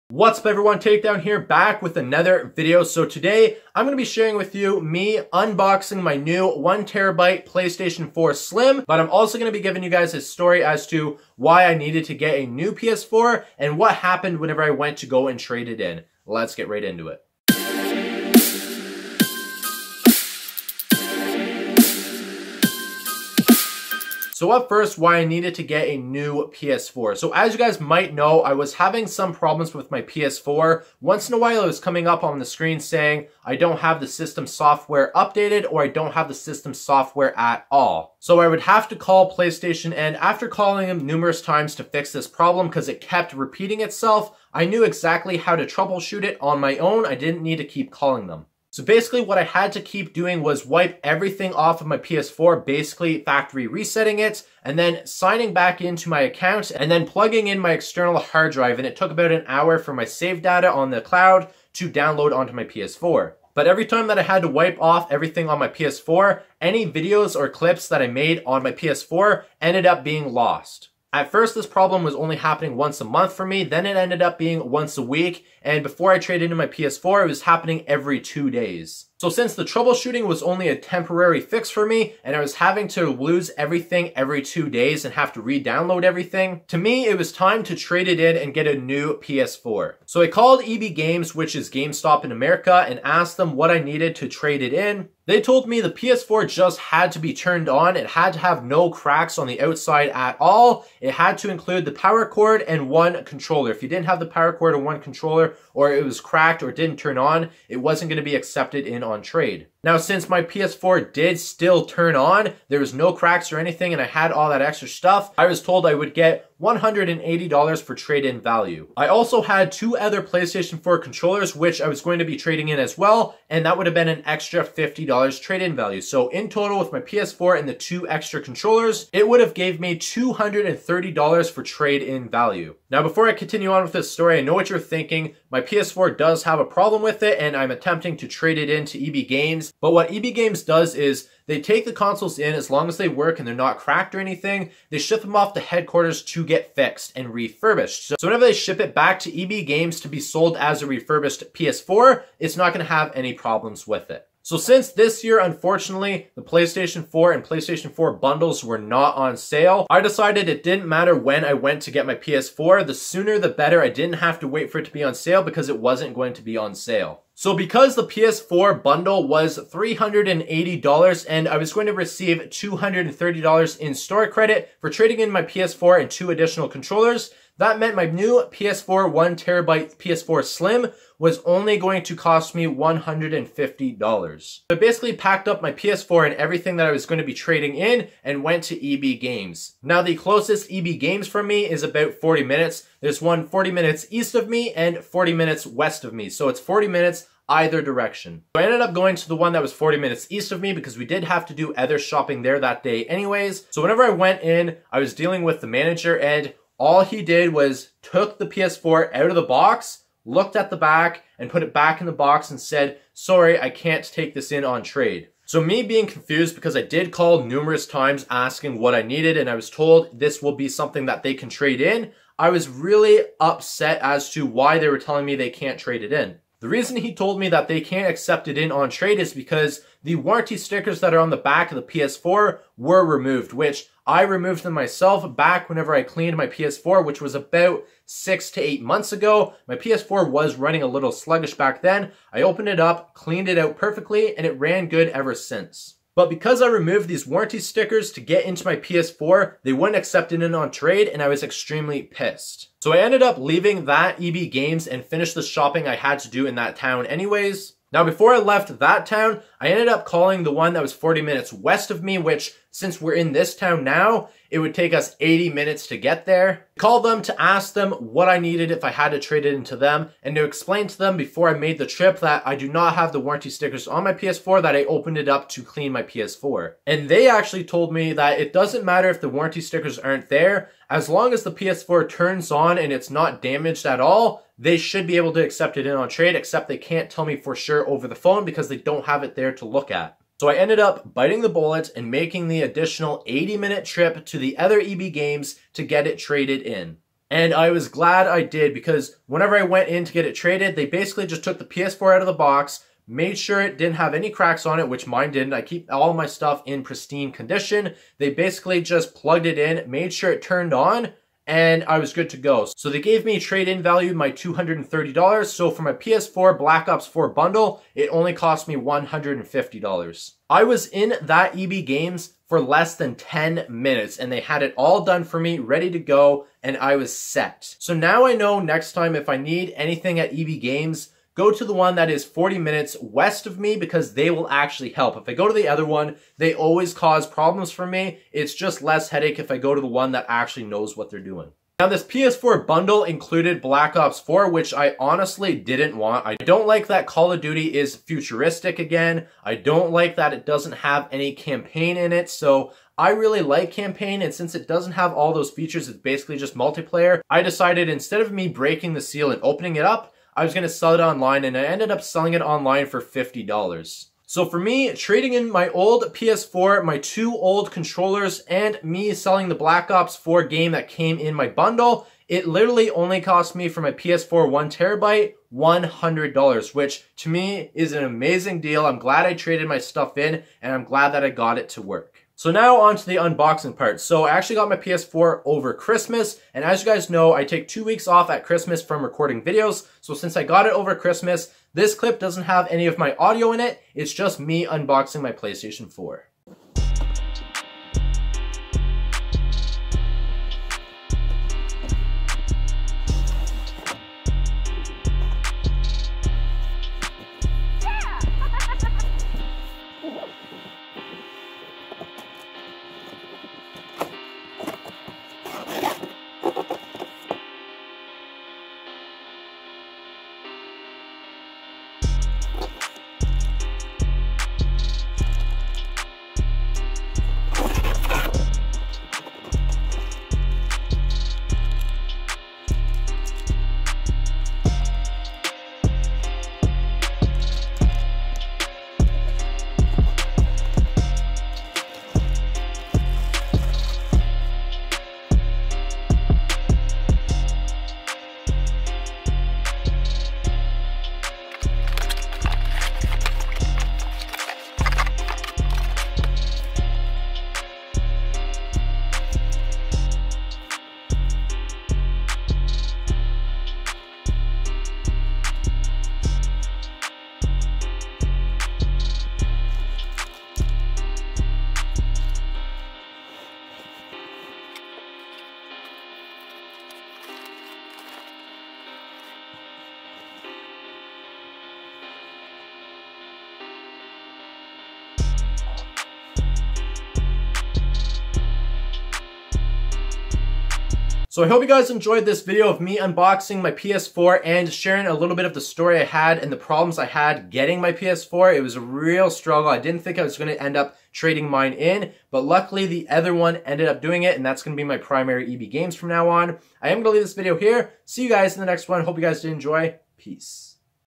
PS4 SLIM 1TB UNBOXING & STORY TIME
I had to get a new PS4, Best part is it only cost me $100 BRAND NEW from EBgames! Today I unbox it and explain how I got it for $100!